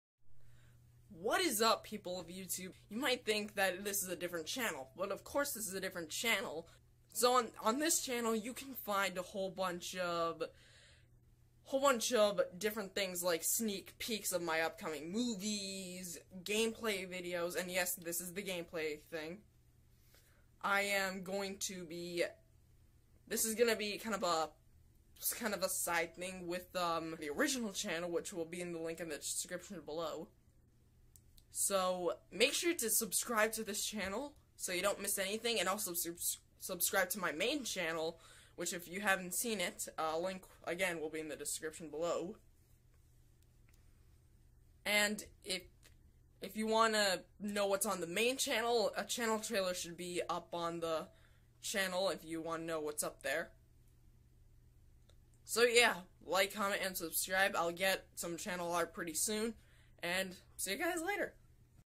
Welcome To My 2nd Channel! | 2nd Channel Reuploads
(Originally uploaded on February 8th, 2018.)
This is the channel trailer to my 2nd channel which has now been closed. This video explains me and my 2nd channel and the things I would have uploaded onto it.